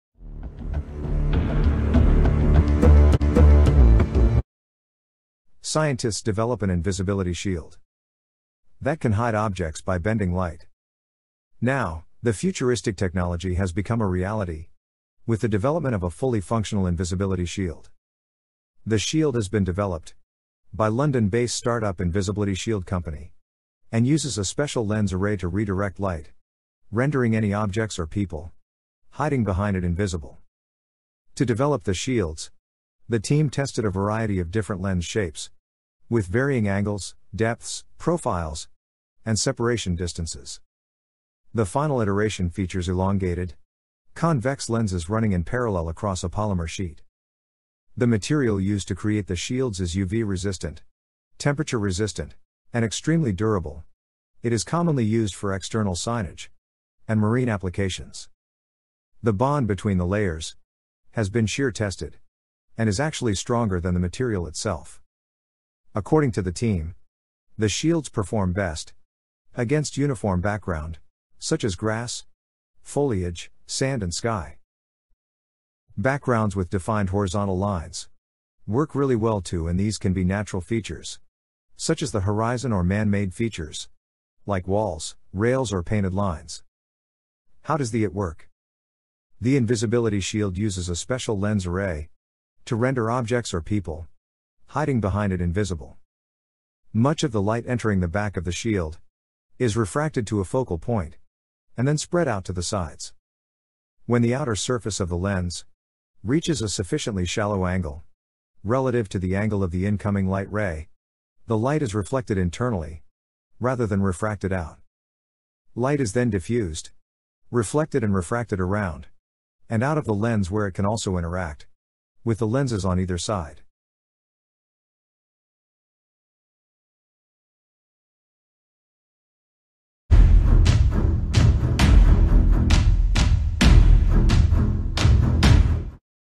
'Invisibility Shield' that can Hide Objects
Now, the futuristic technology has become a reality with the development of a fully functional invisibility shield.
The shield has been developed by London-based start-up 'Invisiblity Shield Co', and uses a special lens array to redirect light, rendering any objects or people hiding behind it 'invisible.'
The invisibility shield uses a special lens array to direct much of the light reflected from the object or person hiding behind it away from the observer, sending it sideways across the face of the shield to the left and right.

IMAGE CREDIT: KICKSTARTER/INVISIBILITY SHIELD CO.